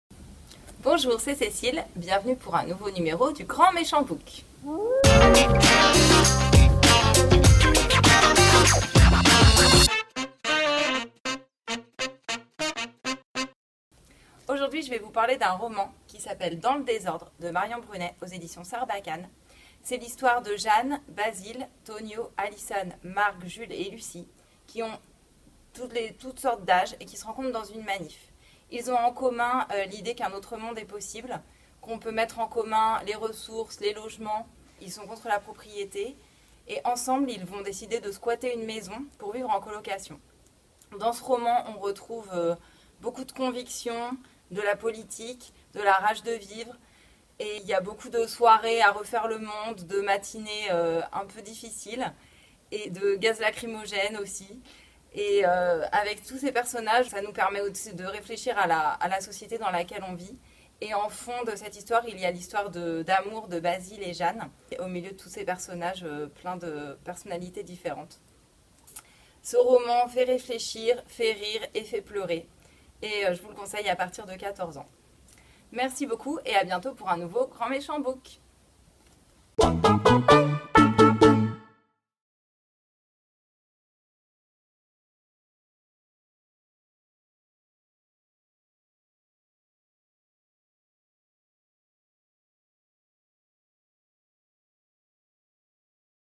Grand Méchant Book #10 - Dans le Désordre - Marion Brunet
Cette semaine, Cécile nous fait découvrir dans ce dixième Booktube, un roman jeunesse « Dans le Désordre » de Marion Brunet aux Éditions Sarbacane.